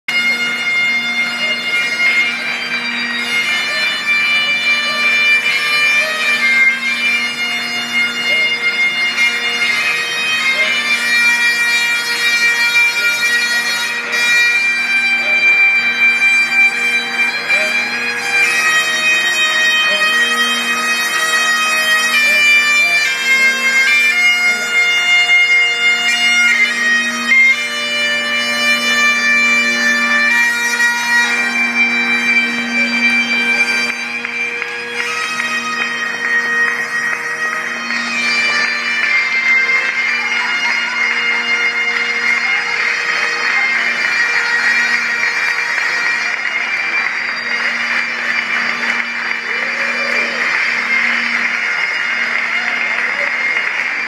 TSA Supports the Honor Flight Network
The members of the TSA Honor Guard and an Austin Police Department bag piper paid tribute to 25 World War II veterans traveling through the Honor Flight Network at Austin-Bergstrom International Airport on May 30, 2014. The Honor Flight Network grants veterans the opportunity to visit memorials in Washington, D.C., built and dedicated to honor their service.